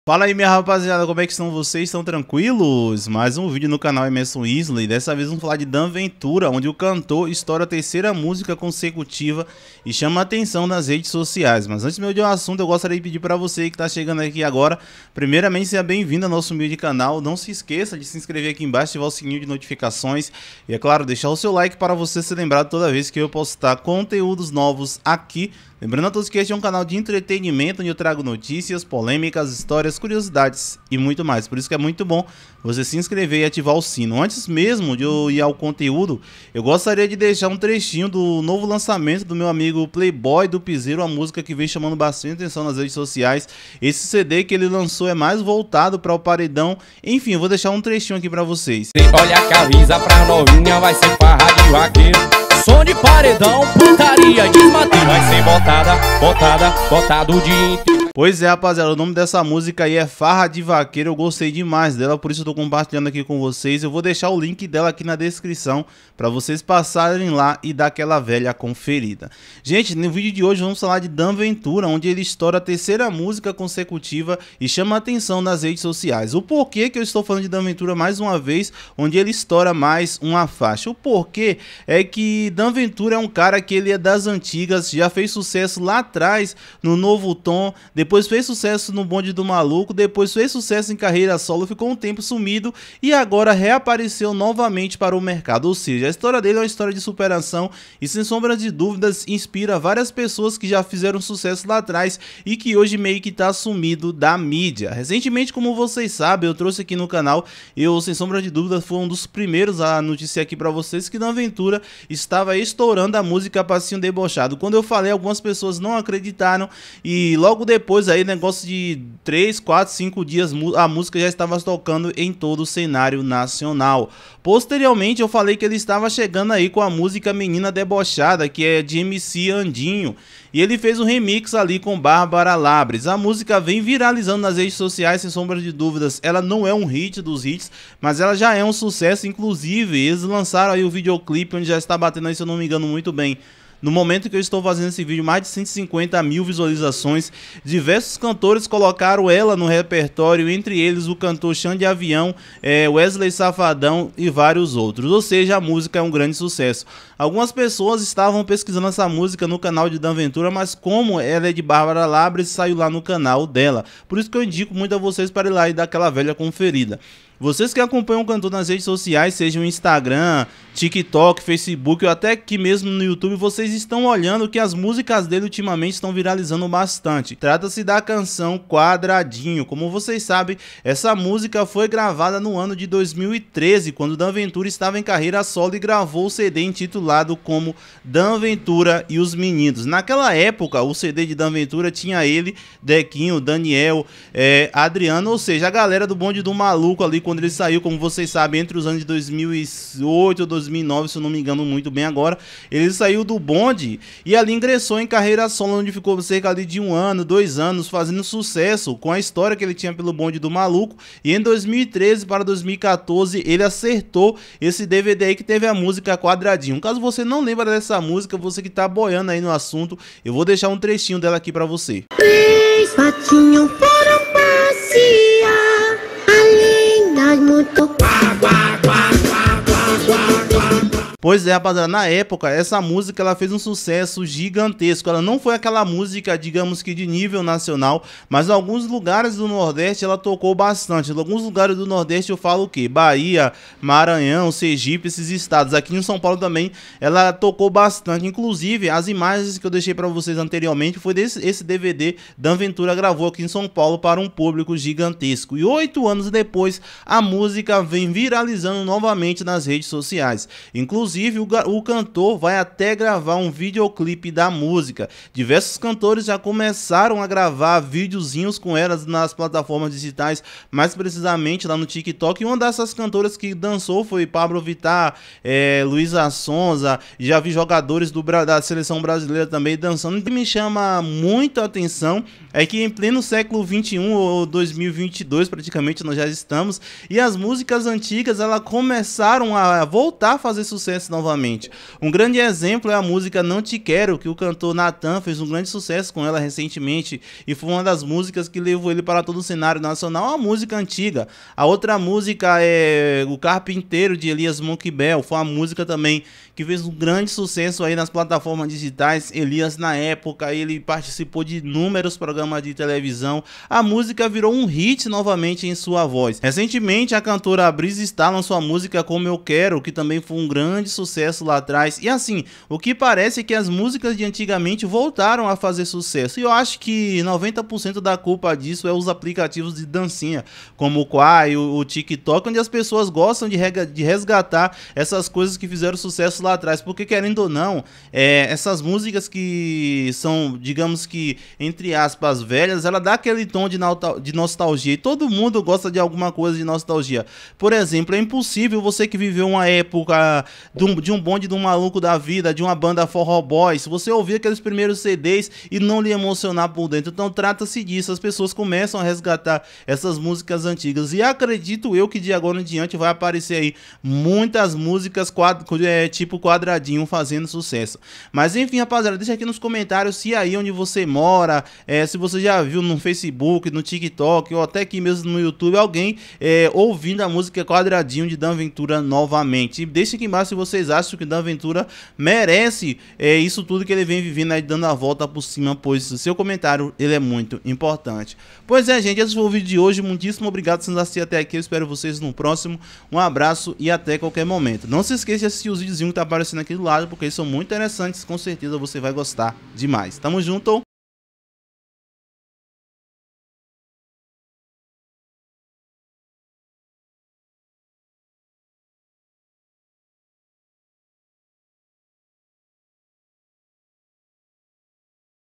DAN VENTURA RESGATA MÚSICA ANTIGA E FAZ SUCESSO
Link da música de Playboy do pizeiro

DAN VENTURA ESTOROU MÚSICA DE 8 ANOS ATRÁS